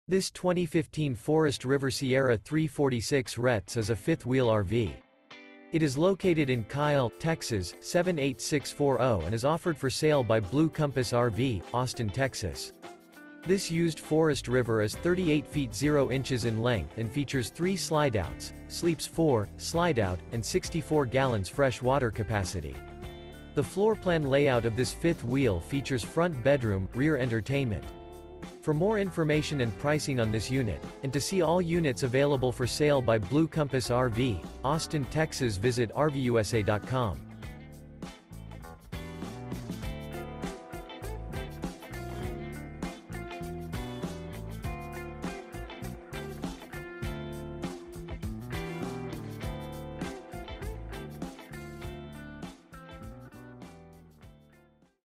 Spectacular 2015 Forest River Sierra Fifth Wheel RV For Sale in Kyle, TX | RVUSA.com
This 2015 Forest River Sierra 346RETS is a Fifth Wheel RV. It is located in Kyle, TX 78640 and is offered for sale by Blue Compass RV - Austin, TX. This Used Forest River Is 38' 0" in length and features 3 slideouts, sleeps 4, Slideout, and 64 gal fresh water capacity. The floorplan layout of this Fifth Wheel features Front Bedroom, Rear Entertainment.